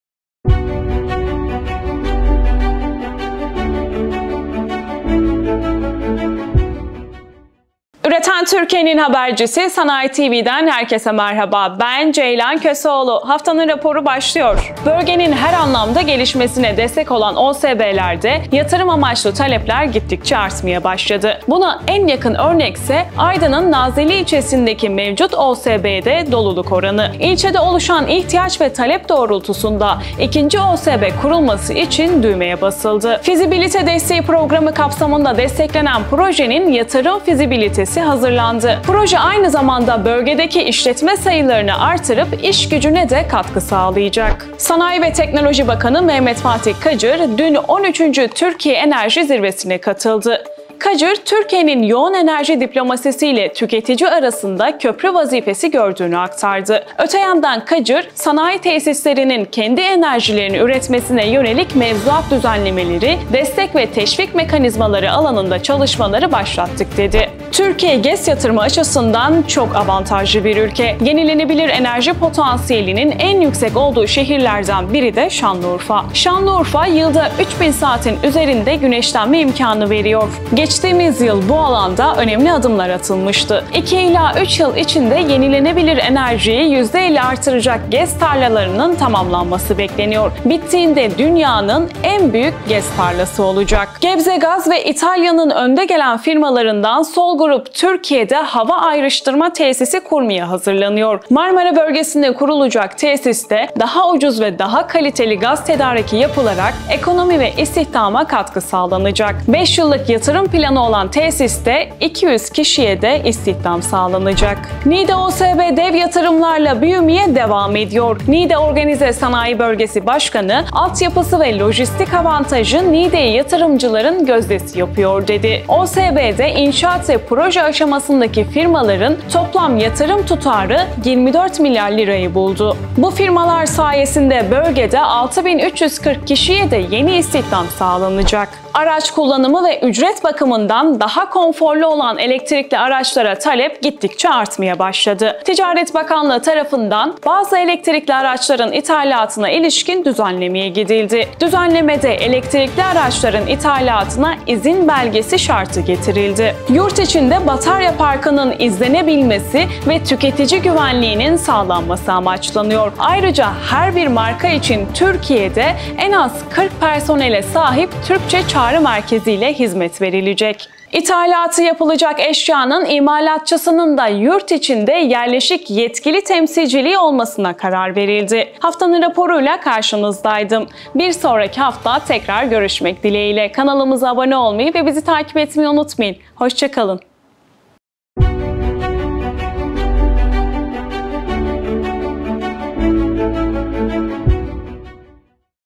Elektrikli Araç İthalatında İzin Belgesi Dönemi...
Haftanın Raporunda Bu Hafta: Yatırımcı Yer Bulamıyor, Türkiye Enerji’de Köprü Vazifesi Görüyor, Dünyanın En Büyük GES Tarlası, Marmara’ya Hava Ayrıştırma Tesisi, Niğde’ye Dev Yatırım, İthalata İzin Belgesi Şartı.
Sanayiye dair yaşanan gelişmeleri sizler için toparladım. Ben Ceylan Köseoğlu Haftanın Raporu’nda gündeme dair en önemli notlar ile karşınızdayım.